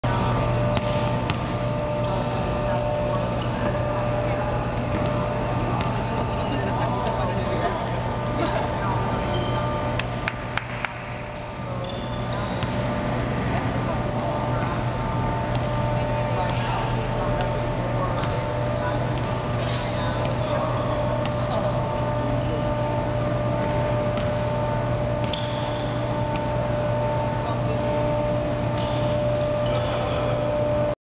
The Boys of AMMEX I-Support- Basketball
Playing some basketball with Scott and Keyo from the USA. Note how they are running and moving faster in this video compared to the one taken a bit later in the scrimmage.